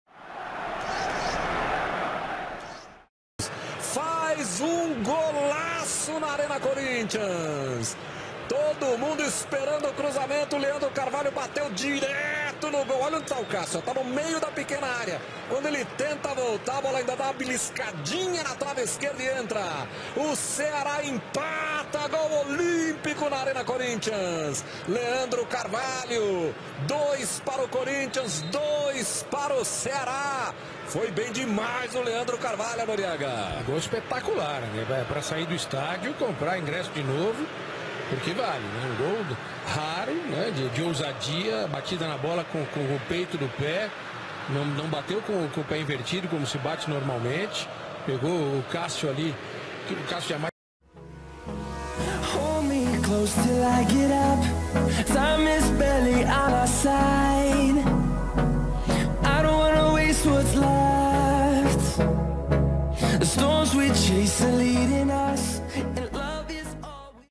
El espectacular gol olímpico en el último minuto de partido en Brasil ⚽ CORINTHIANS ⚽ 2019
🔸 Sucedió en el Corinthians contra el Ceará. Leandra Carvalho lanzó el córner con toda la intención del mundo. Para ver una y otra  vez.